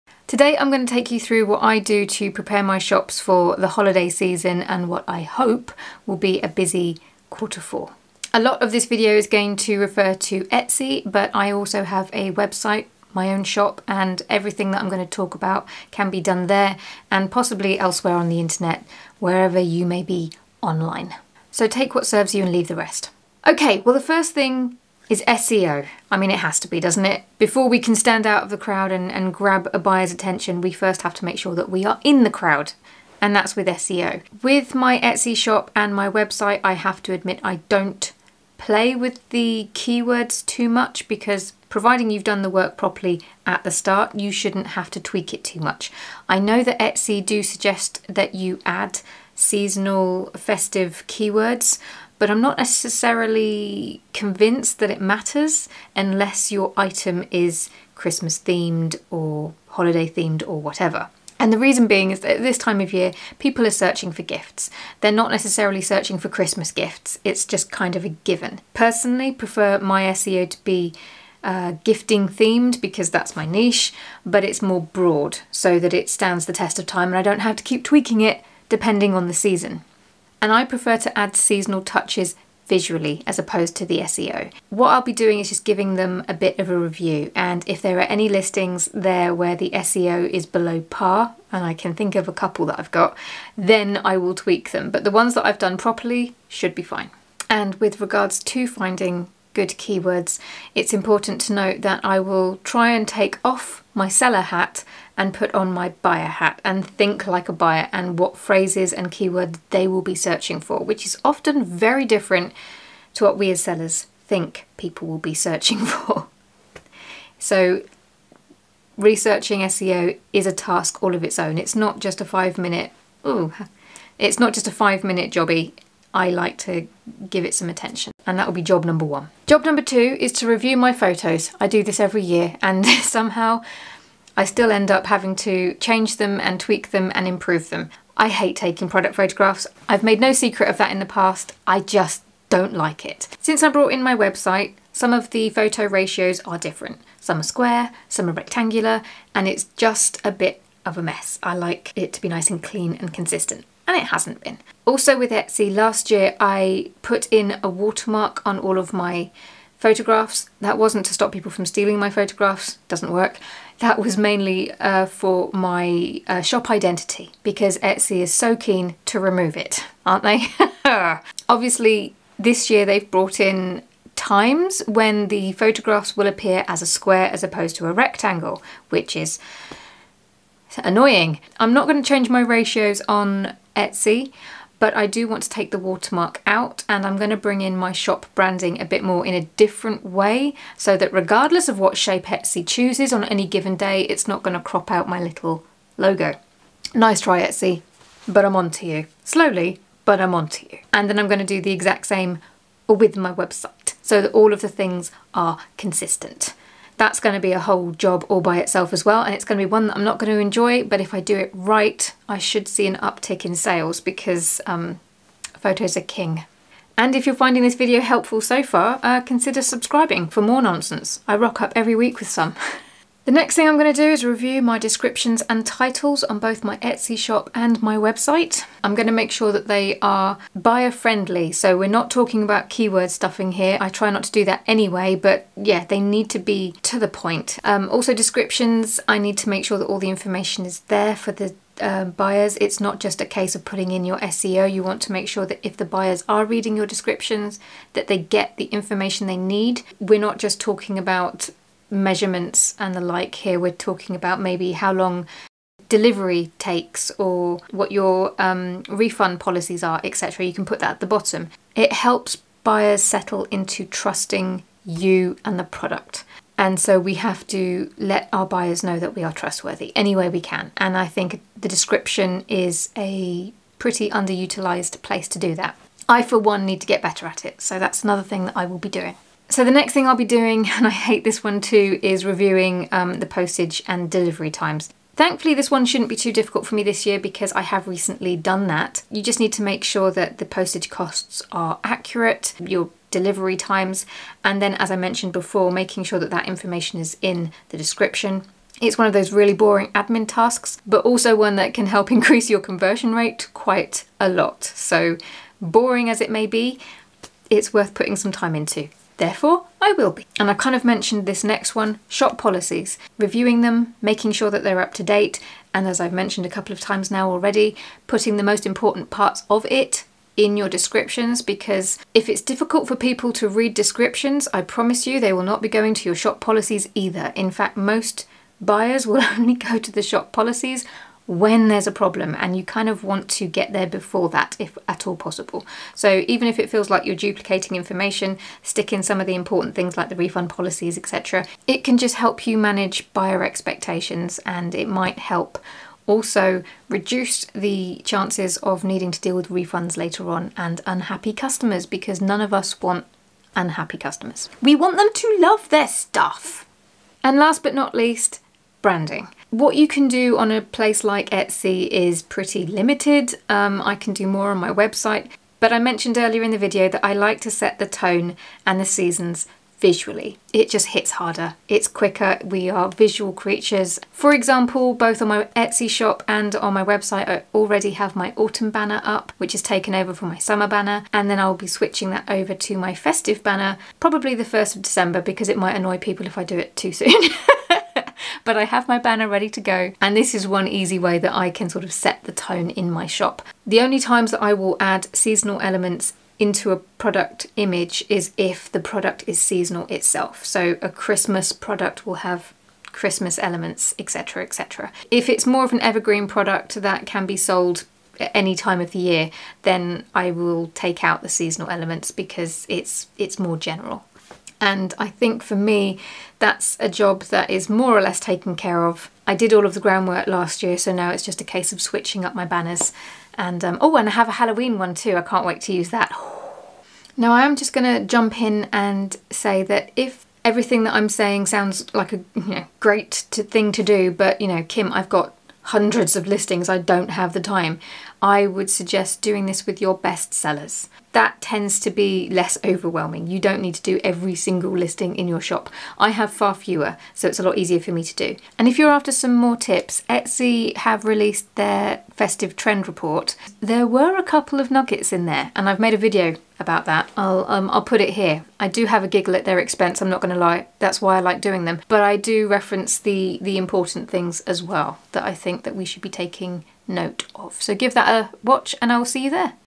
Get Your Etsy Shop Ready For Q4! 🎄🎁
In this video, I'm sharing tips on how I get my Etsy and website shops ready for the holiday season.
If you're a shop owner on Etsy, then you know that the holidays are an important time of the year. From SEO to photographs to shipping and all things in between, I'll show you everything I cover to make my shop a success during the festive period!